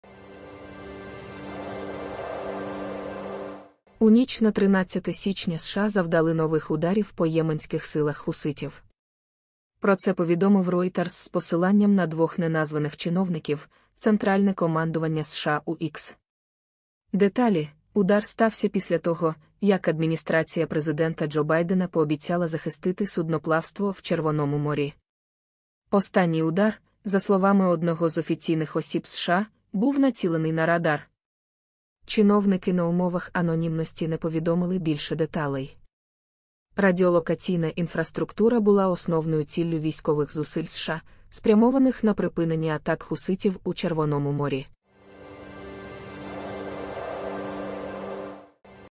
США повторно завдали ударів по хуситах у Ємені
USDT: TSJfcHW5UBth2qsR6jYUeGsxJDYYL16YCL

Bitcoin
BTC: 1PYFjXGCBvzyjpbuPRiy1P7DwndWNcTKfG